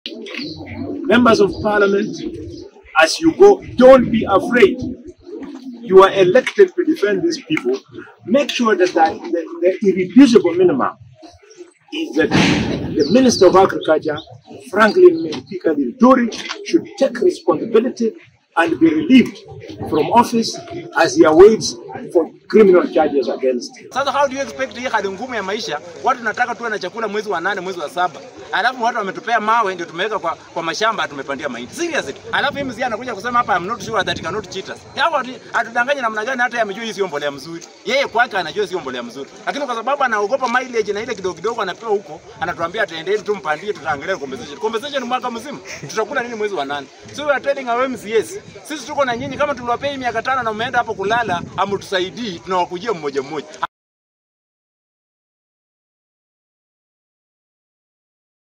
MPs collect samples to investigate fertilizer syndicates.
The Member of Parliament for Nyando constituency Jared Okelo together with his Matayos Counterpart Geoffrey Makokha Odanga have visited Kakamega NCPB depot on a fact-finding mission on the alleged distribution of substandard fertiliser.